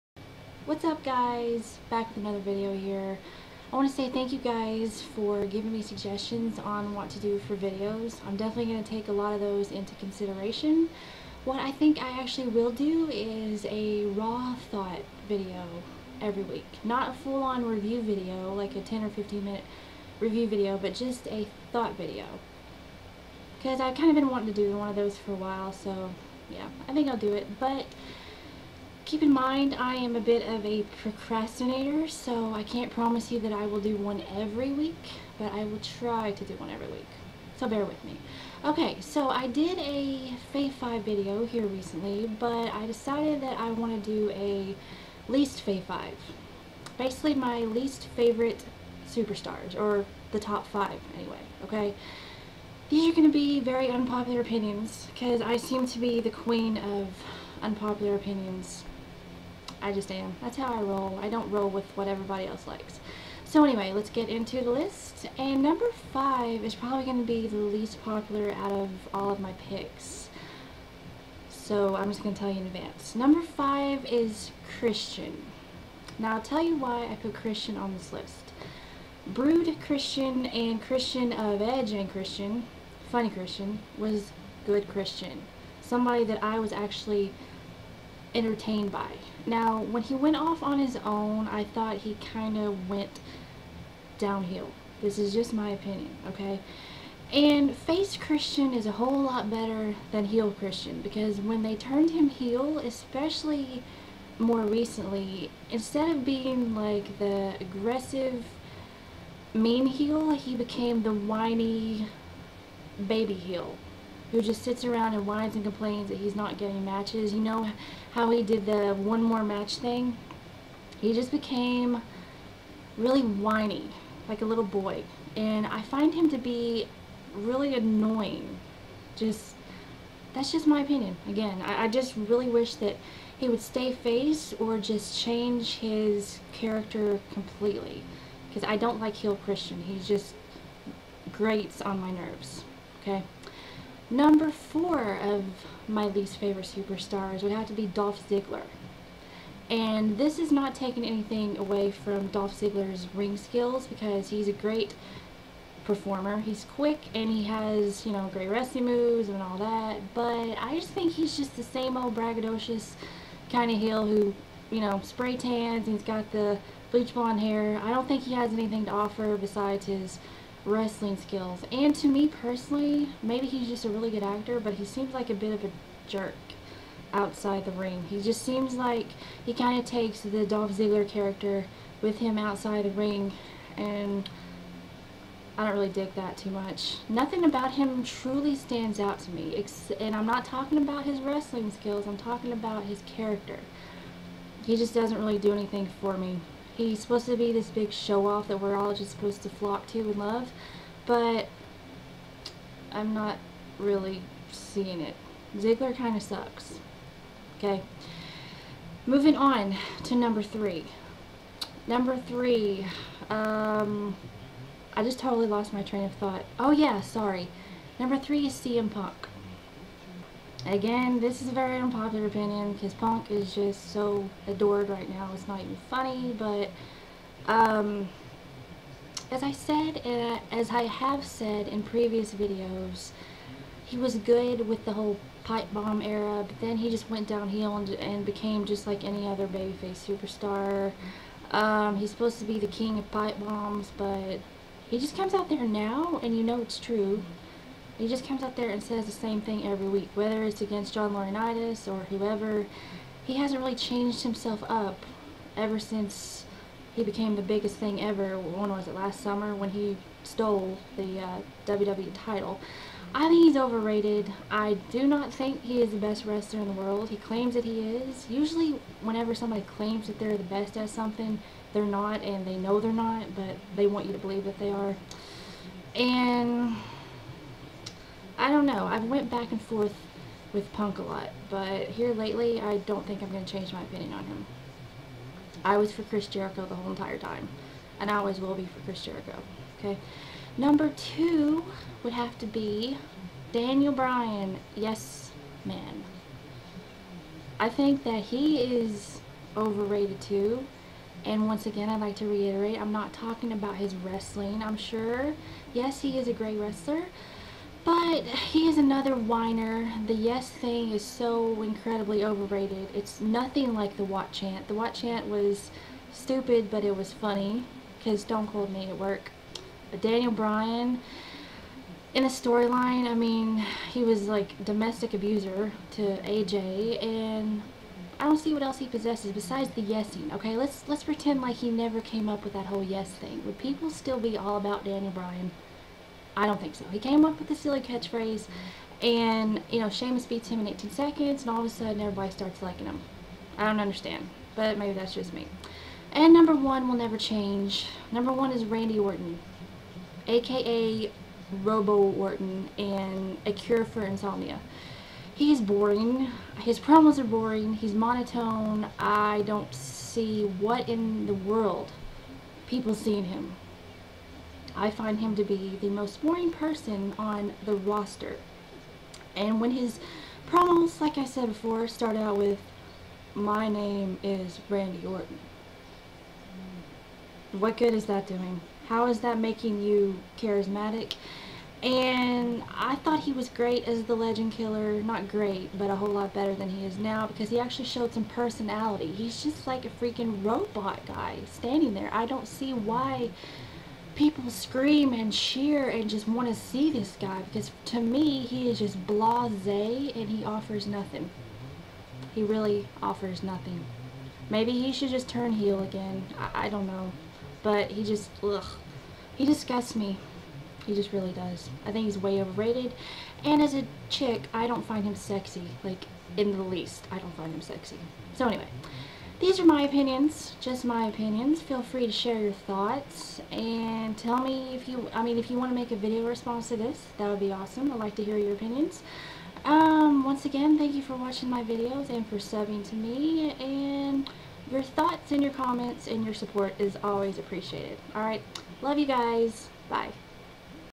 My Least Fave Five (More Unpopular Opinions, Yes!)
Giving you the rundown on who I basically can't stand haha!